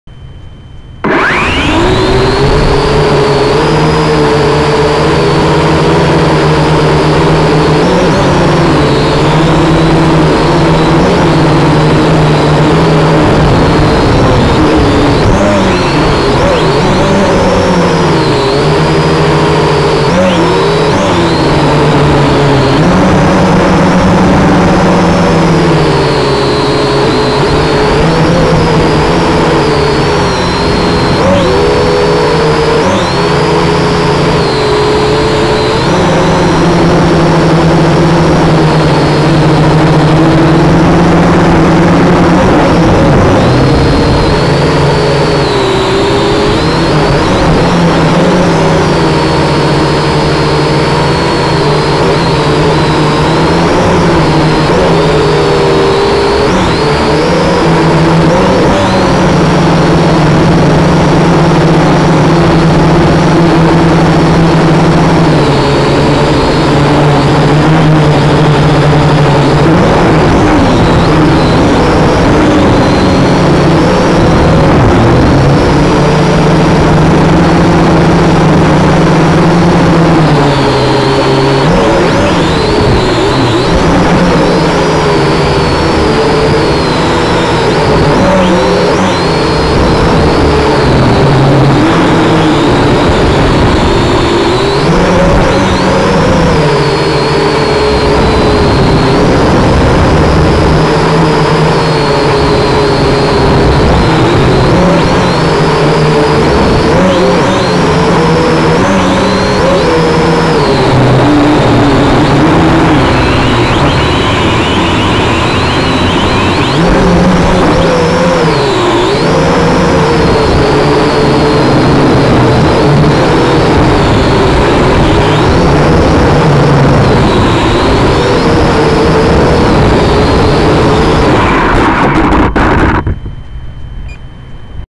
Kaiser Baas Alpha Drone Flight #2
A longer flight this time around...and no crash!